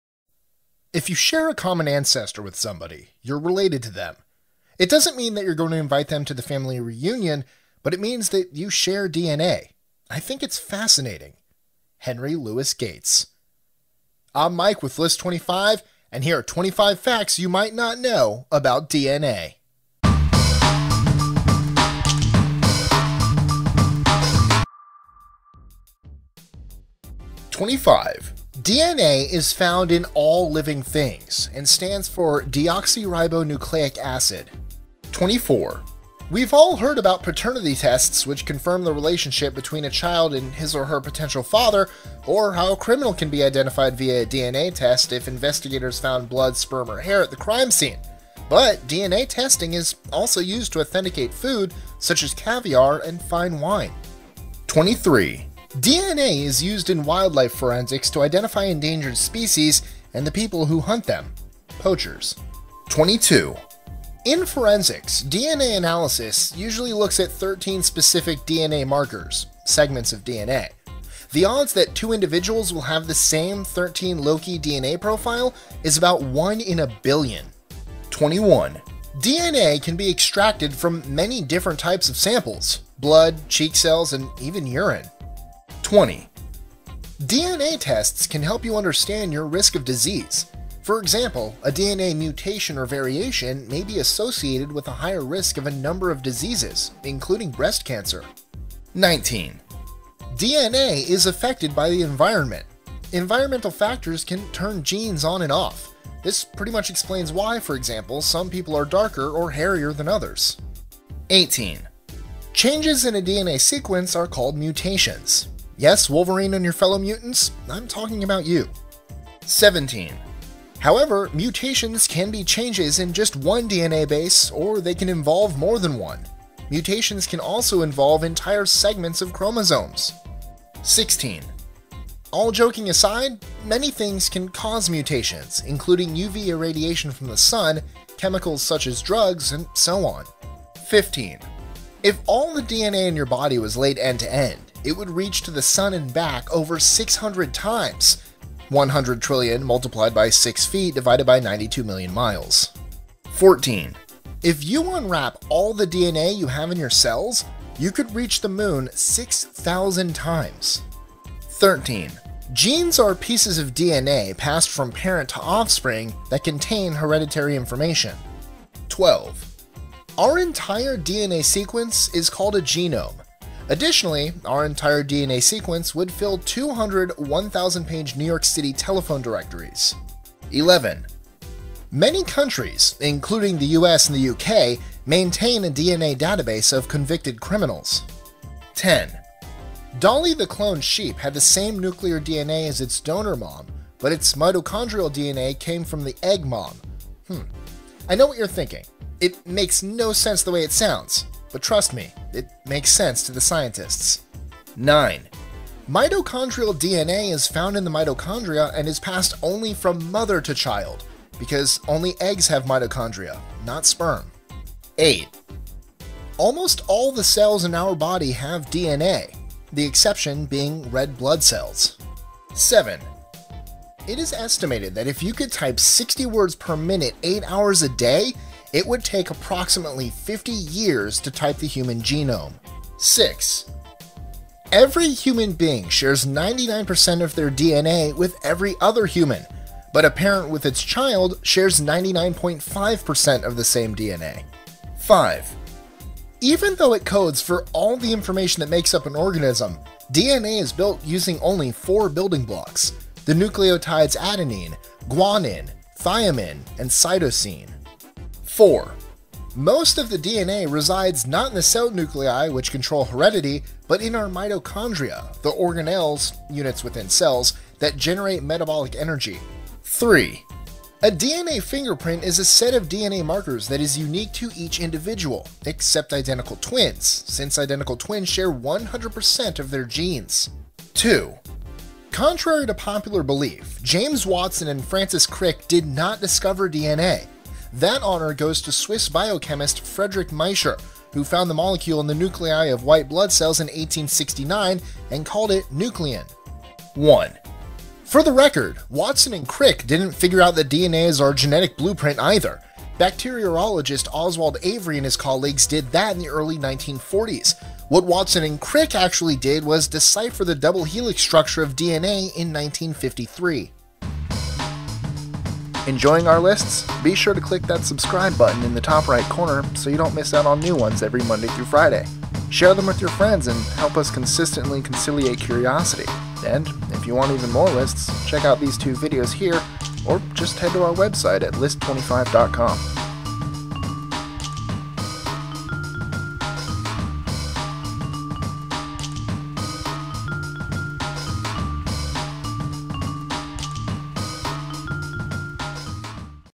25 Facts You Might Not Know About DNA
DNA is a long molecule that contains our unique genetic code and thus the instructions an organism needs to develop, live, and reproduce. Just like a recipe book, it holds the directions for making all the proteins in our bodies. This information is found in every cell and is passed down from parents to their children. DNA has become particularly popular during the past few decades thanks to its broad usage and utility. Genetic tests are used for a variety of reasons nowadays, including to show if there’s a relation between a parent and a child, diagnose genetic disorders, to determine whether a person is a carrier of a mutation that they could pass on to their children, and to identify whether a person is at risk for a certain disease. For the aforementioned reasons, it has been argued that the discovery of DNA as well as our understanding of its structure and function may well be the most significant discovery of the twentieth century. The effect of the discovery of DNA on scientific and medical progress has been enormous! See for yourself as we explore these 25 Facts You Might Not Know About DNA.

DNA is found in all living things and stands for deoxyribonucleic acid.

We have all heard about paternity tests which confirm the relationship between a child and his or her potential father, or how a criminal can be identified via a DNA test (if investigators found blood, sperm, or hair at the crime scene), but DNA testing is also used to authenticate food such as caviar and fine wine.

DNA is used in wildlife forensics to identify endangered species and the people who hunt them (poachers).

In forensics, DNA analysis usually looks at thirteen specific DNA markers (segments of DNA). The odds that two individuals will have the same thirteen-loci DNA profile is about one in a billion.

DNA can be extracted from many different types of samples: blood, cheek cells and even urine.

DNA tests can help you understand your risk of disease. For example, a DNA mutation or variation may be associated with a higher risk of a number of diseases, including breast cancer.

DNA is affected by the environment; environmental factors can turn genes on and off. This pretty much explains why, for example, some people are darker or hairier than others.

Changes in a DNA sequence are called mutations. Yes, Wolverine and your fellow mutants, we’re talking about you!

However, mutations can be changes in just one DNA base or they can involve more than one. Mutations can also involve entire segments of chromosomes.

All joking aside, many things can cause mutations, including UV irradiation from the sun, chemicals such as drugs, and so on.

And more...